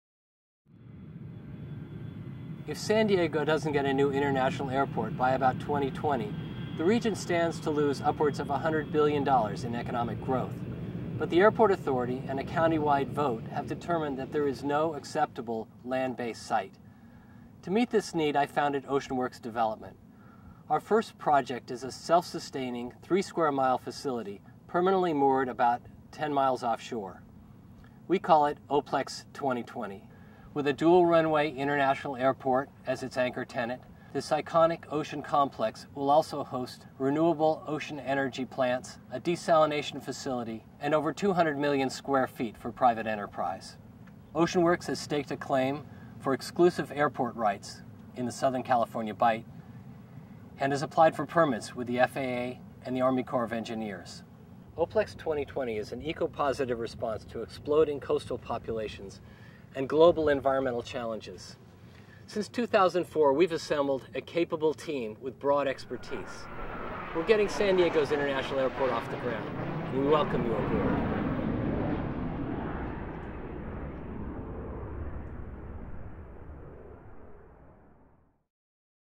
O-Plex 2020
Self-sustaining, three (3) square mile, permanently moored ocean- based infrastructure facility to host San Diego's regional International Airport, renewable ocean energy extraction, production and transmission from waves, wind, ocean currents, tides, thermal gradients (Ocean thermal energy conversion or OTEC), salinitiy gradients and biomass from algae; and other mariculture for fish farming, restoration of marine fisheries; ocean clean-up.  Facility will have hotels, retaurants, convention centers, casinos, high end retail malls, commercial and office facilities, shipping and distribution facilities, free trade zones,